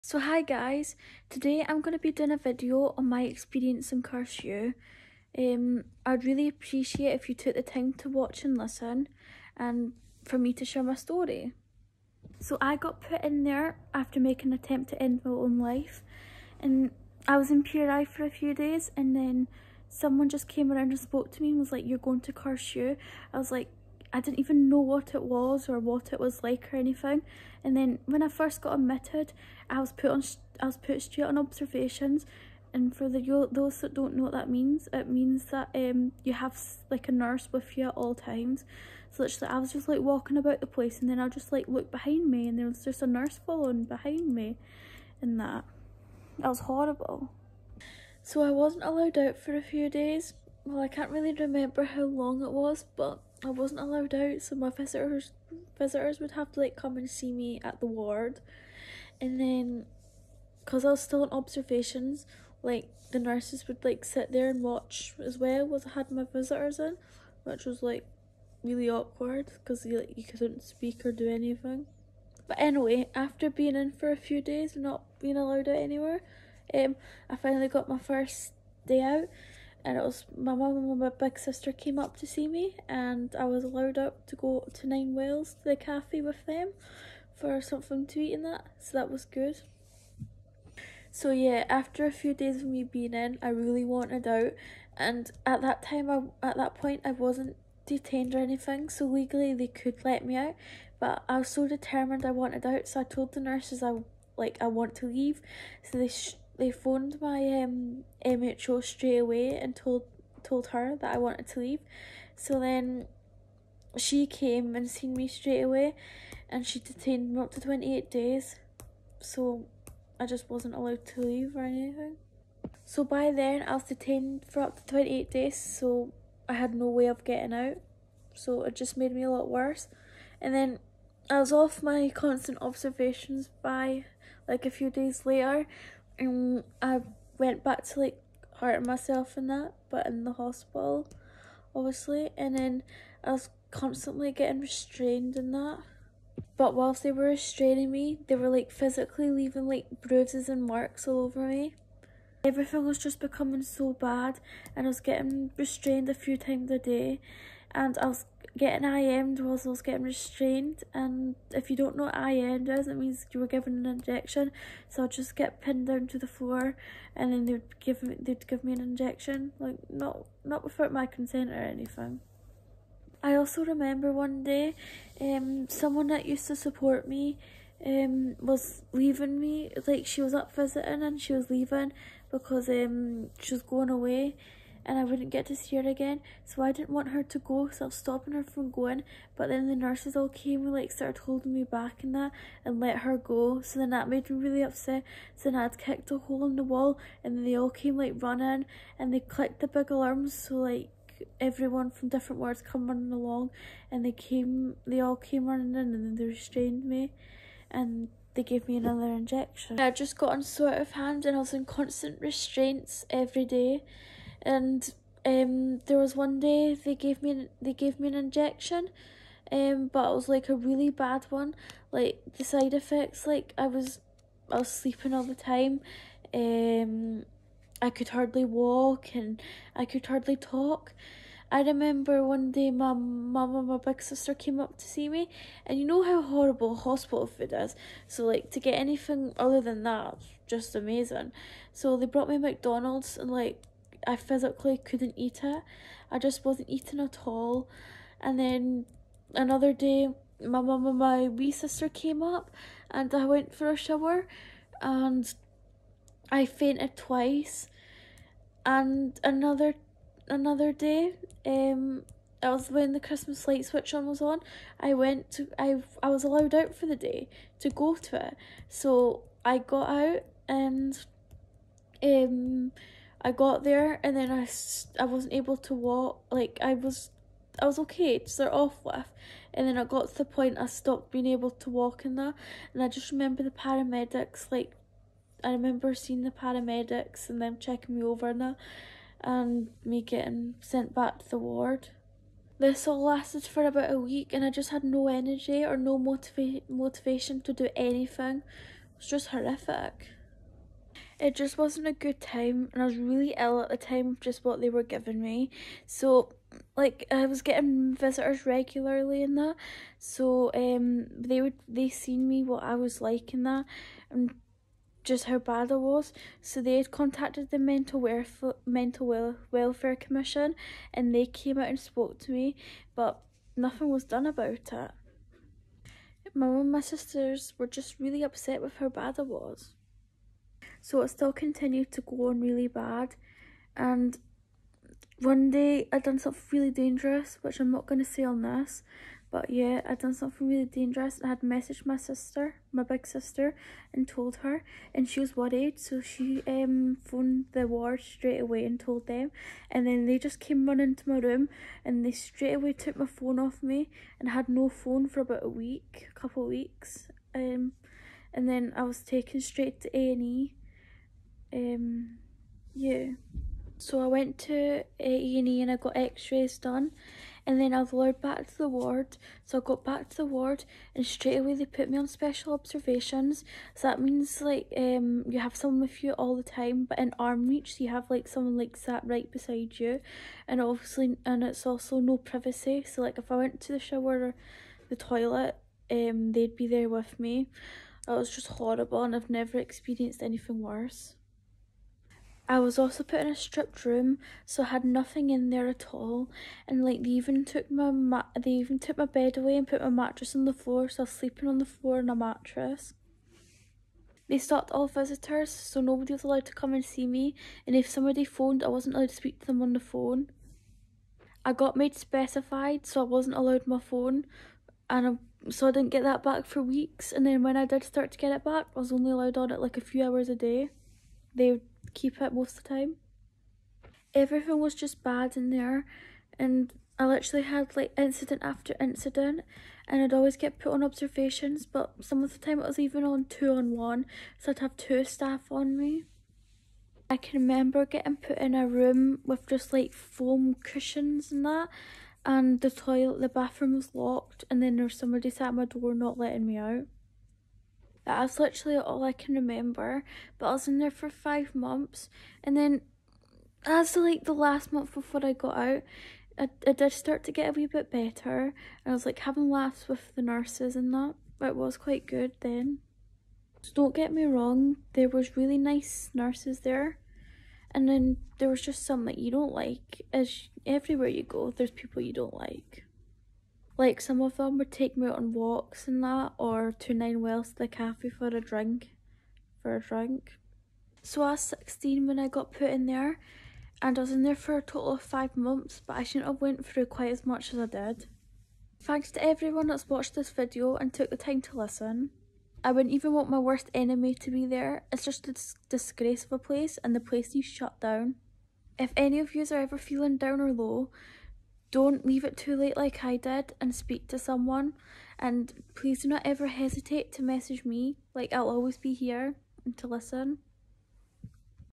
My experience in carseview
This is just a wee video of my experience in carseview, if your easily triggered don’t listen, but I’d hope you would listen to my story and if your ever feeling low don’t hesitate to message me 🙂